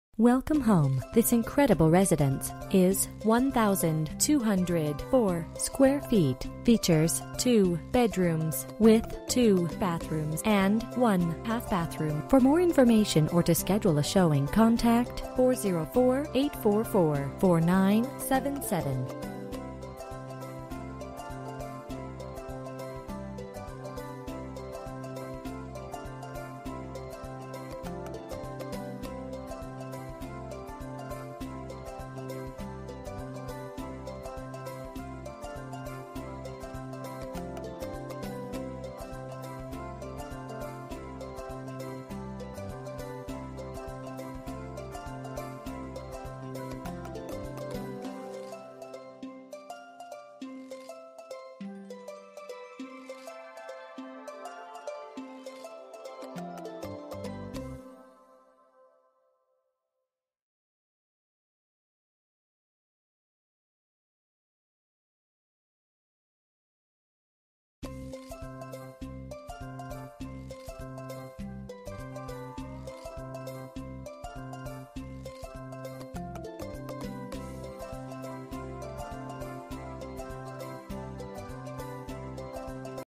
Residential for sale - 3240 Long Iron Place, Lawrenceville, GA 30044
Come check out this cute townhome. Located in a quiet community. It offers 2 bedrooms & 2.5 baths. One  year old roof, hardwood floors on main level, SS appliances & comes with Nest system.

Bedrooms: 2
Bathrooms: 2 full and 1 half
Square Feet: 1204
Price: $145,900